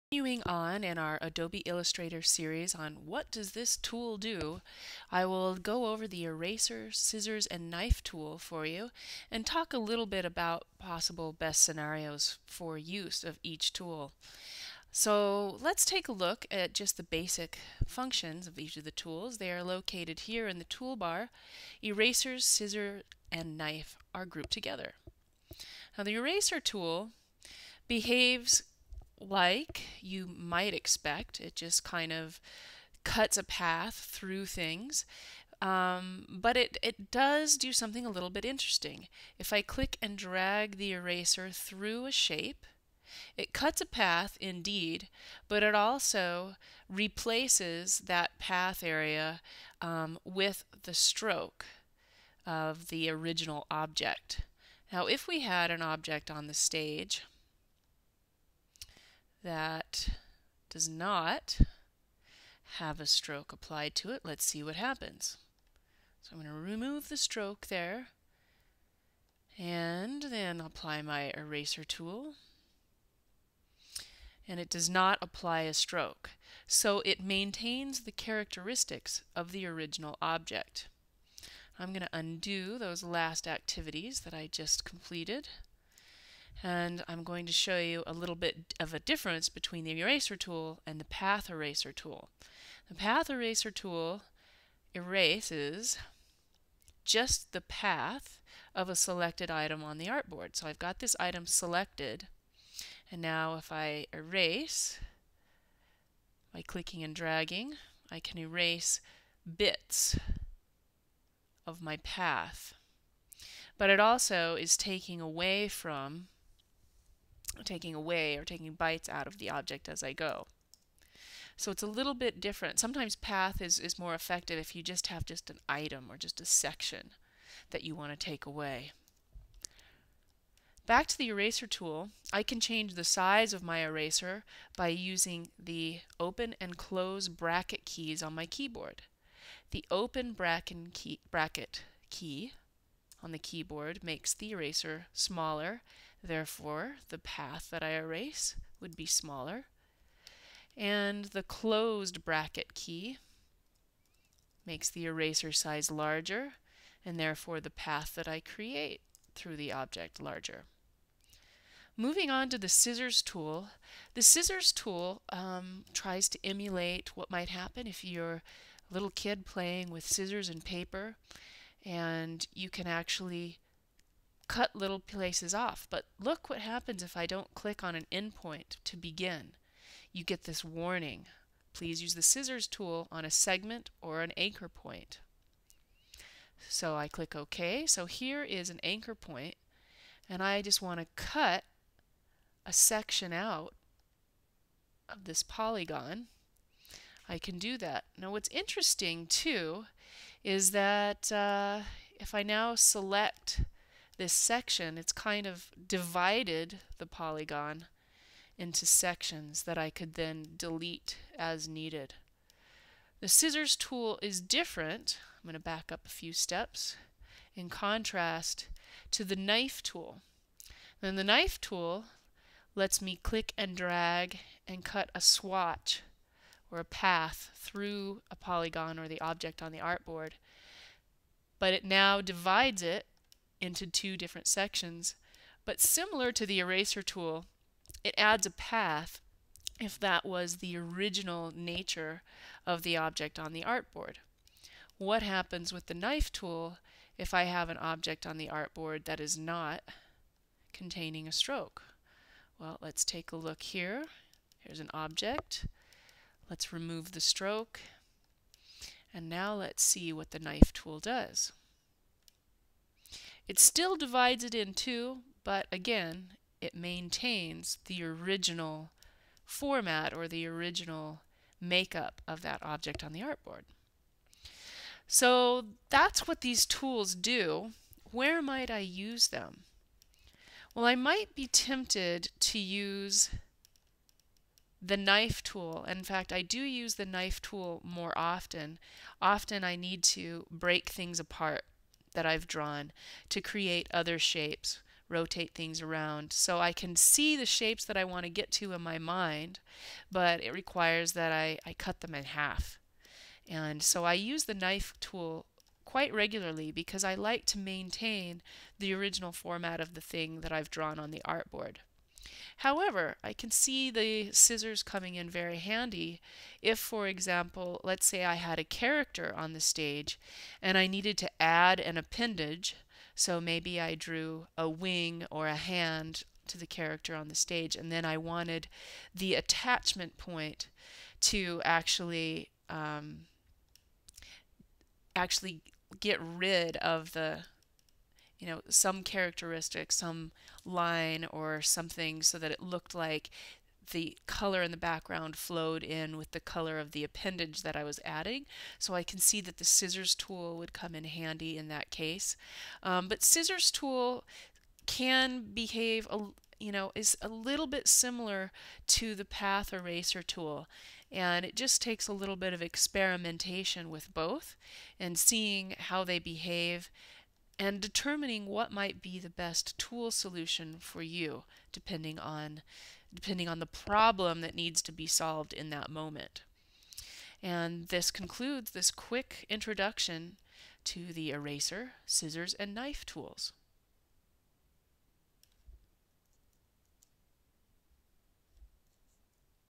Adobe Illustrator CS6 Eraser, Scissors, Knife tools
Introduction to working with Eraser, Scissors and Knife tools in Adobe Illustrator.